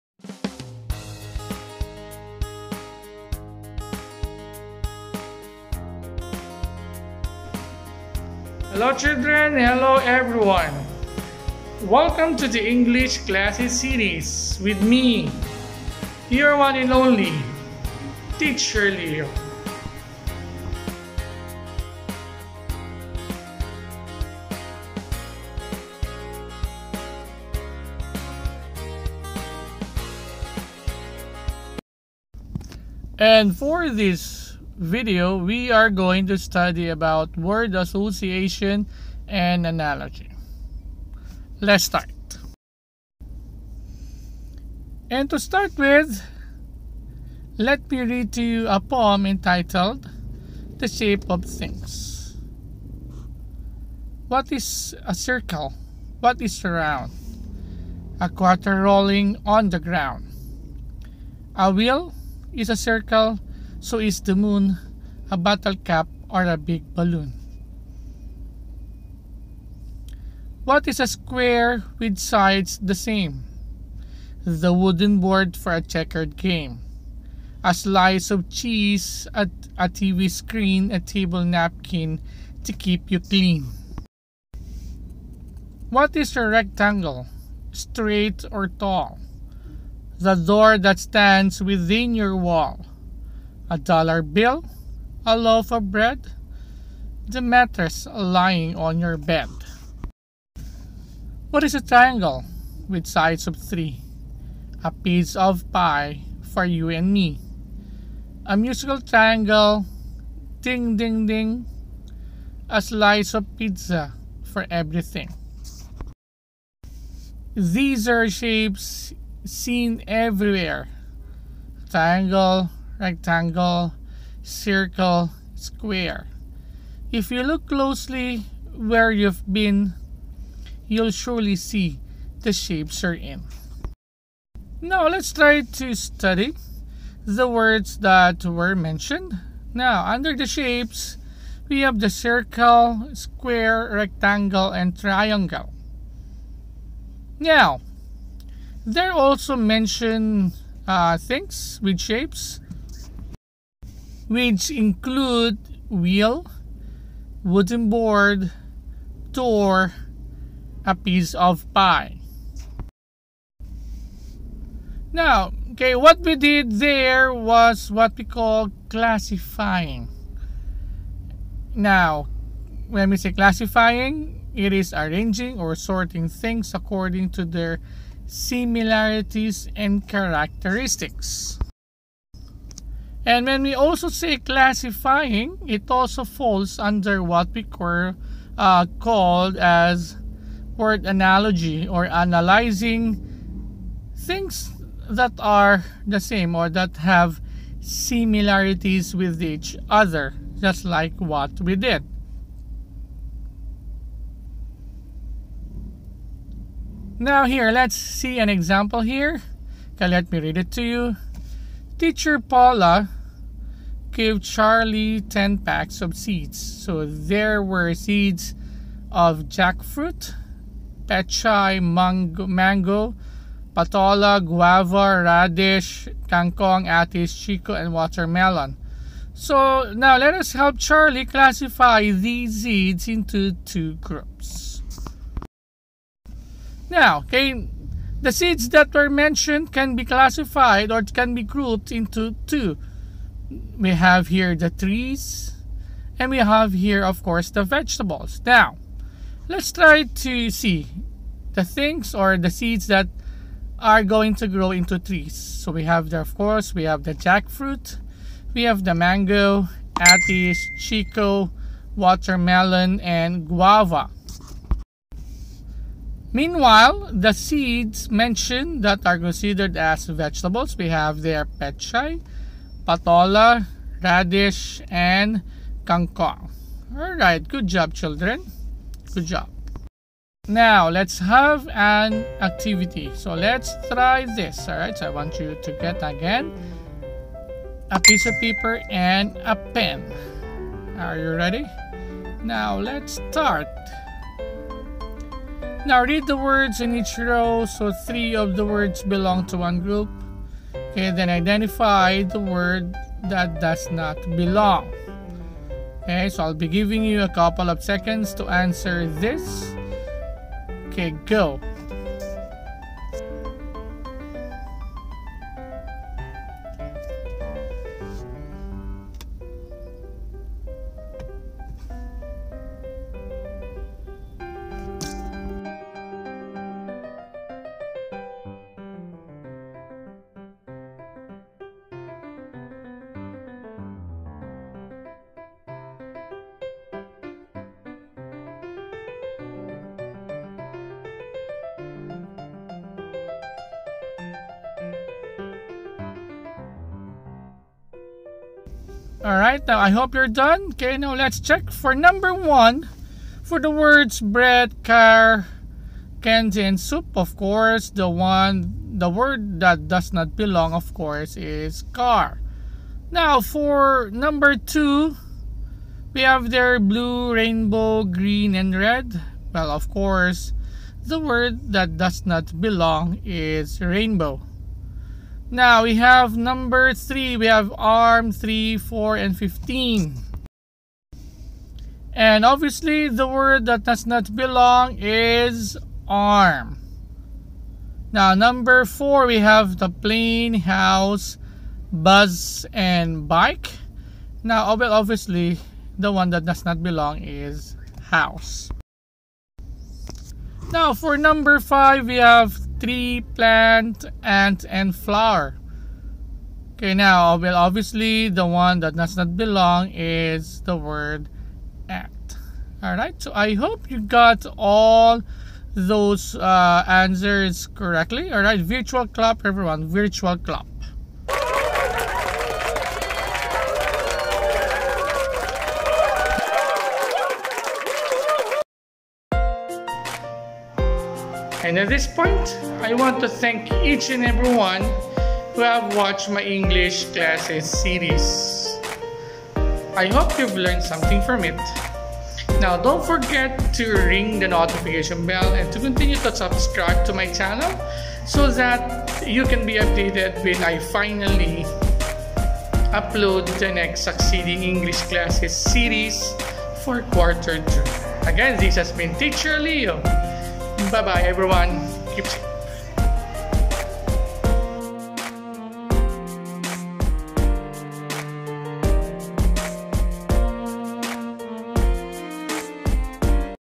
English Classes Series with Titser Leo - Word Association and Word Analogy
Getting the meaning of words through word classification and word analogy